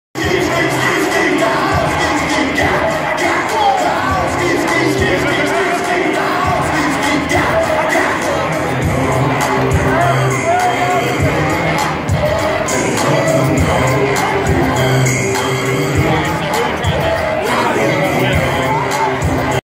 Heather riding the mechanical bull in Atlanta.
Heather riding the mechanical bull in Atlanta. Skeet skeet